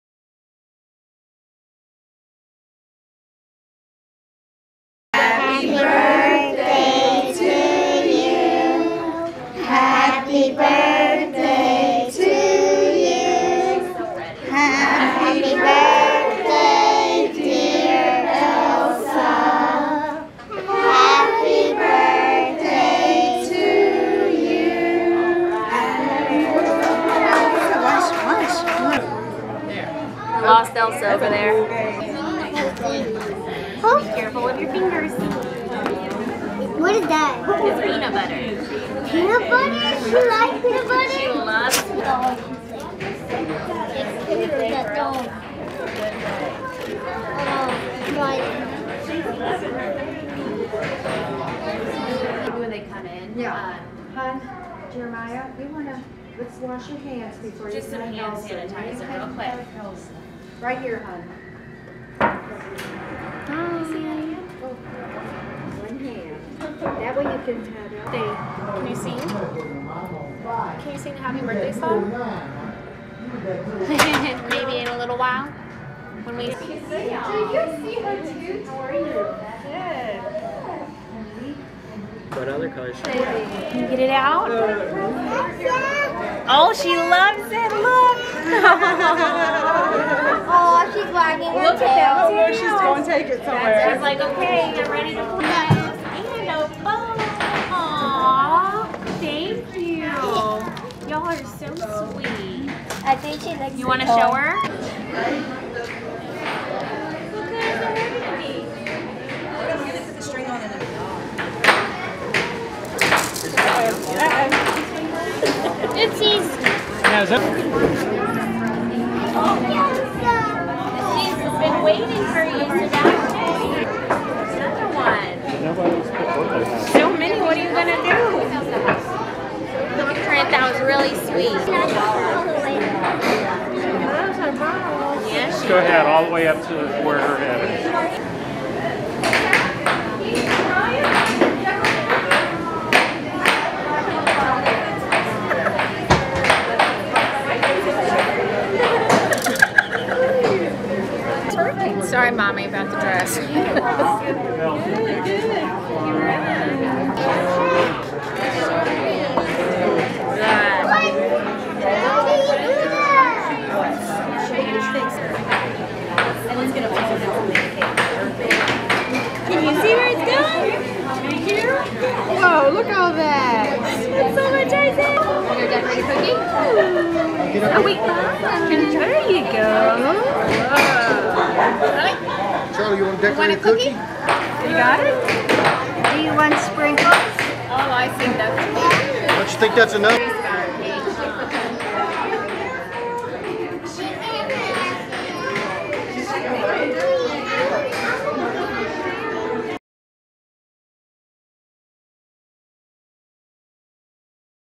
VIDEO: Texas Children's Hospital holds birthday bash for therapy dog
Elsa's birthday